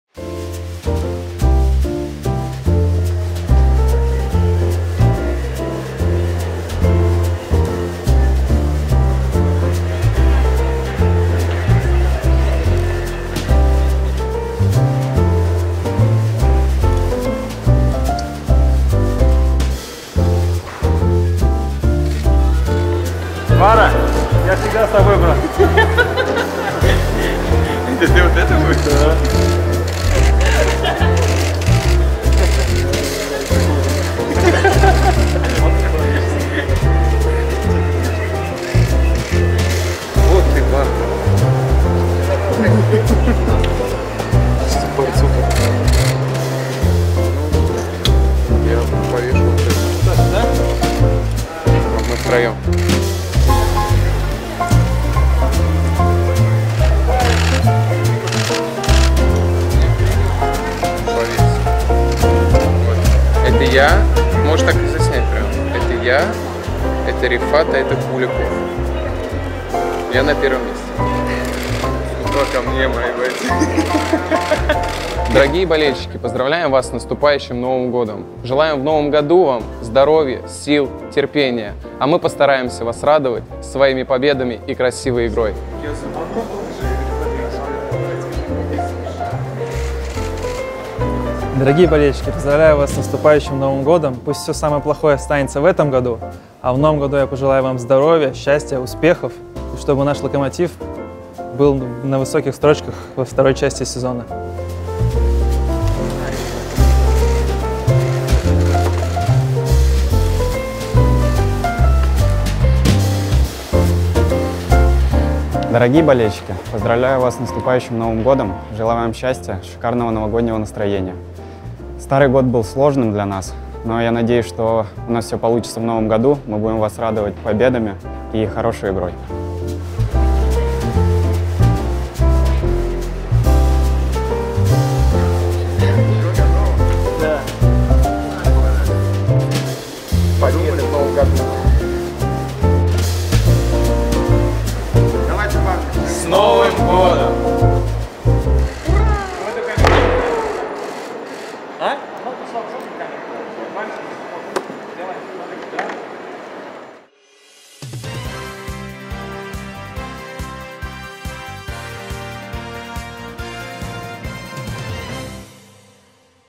«Локомотив» поздравляет с Новым годом!
Дима Баринов, Даня Куликов и Рифат Жемалетдинов прогулялись по новогодней ярмарке Bosco, вместе нарядили ёлку и собрали именные подарки, которые мы совсем скоро разыграем среди болельщиков «Локо».
Делимся тёплыми пожеланиями и поздравляем с наступающим Новым годом в нашем немножко волшебном видео.